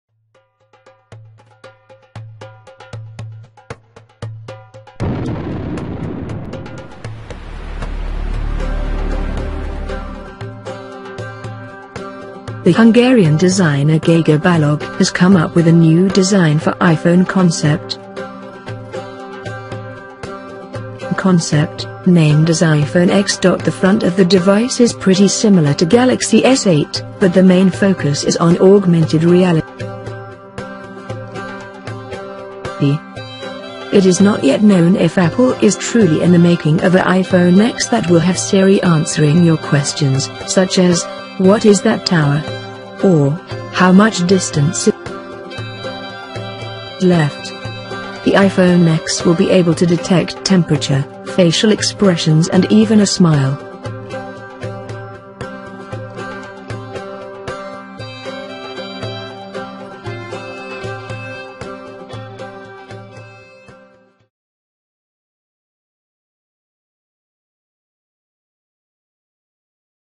Apple iPhone X Augmented Reality Smartphone Wide Screen-Curved OLED
The Hungarian designer Gagor Balogh has come up with a new design for iPhone concept, named as iPhone X.The front of the device is pretty similar to Galaxy S8, but the main focus is on Augmented Reality.

It is not yet known if Apple is truly in the making of a iPhone X that will have Siri answering your questions, such as, what is that tower? Or, how much distance is left?

The apple iphone X will be able to detect temperature, facial expressions and even a smile.